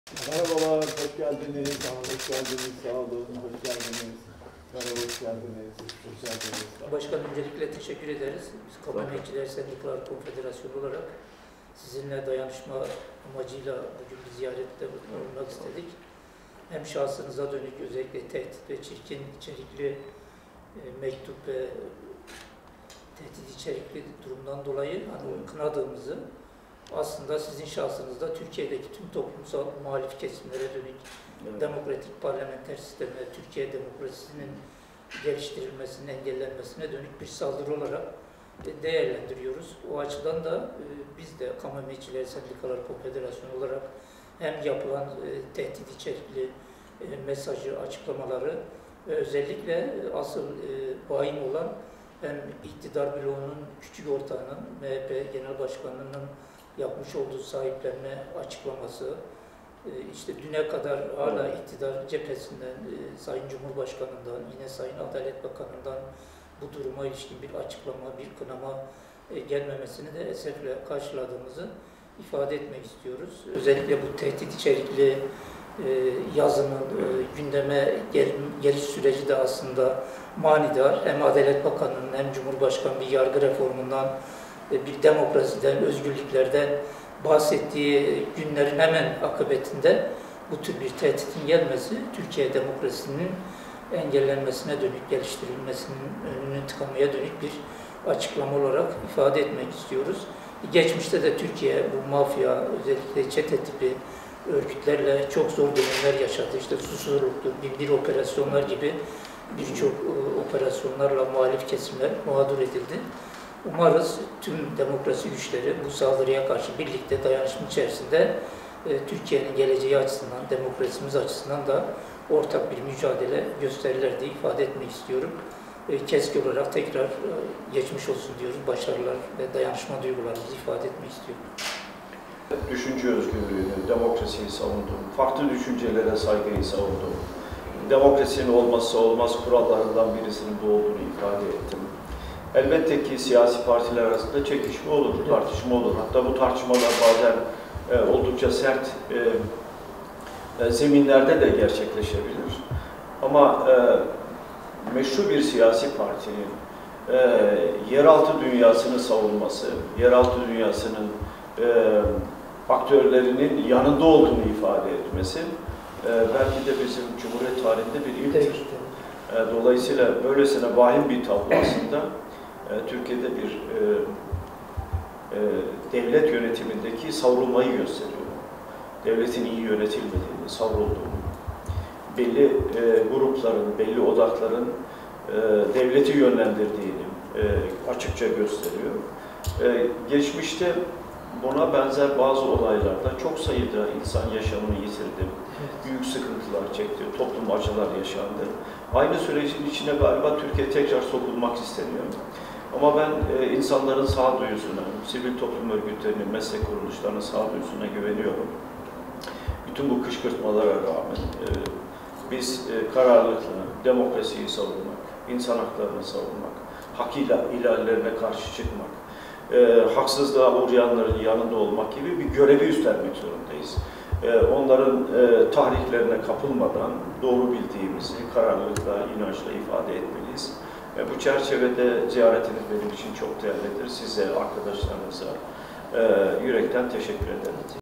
CHP GENEL BAŞKANI KEMAL KILIÇDAROĞLU KESK HEYETİNİ KABUL ETTİ 20/11/2020
Cumhuriyet Halk Partisi Genel Başkanı Kemal Kılıçdaroğlu, KESK Eş Genel Başkanı Mehmet Bozgeyik ve beraberindeki heyeti, CHP Genel Merkezi'ndeki makamında kabul etti. Görüşmede CHP lideri Kılıçdaroğlu’na Genel Başkan Yardımcısı Veli Ağbaba eşlik etti.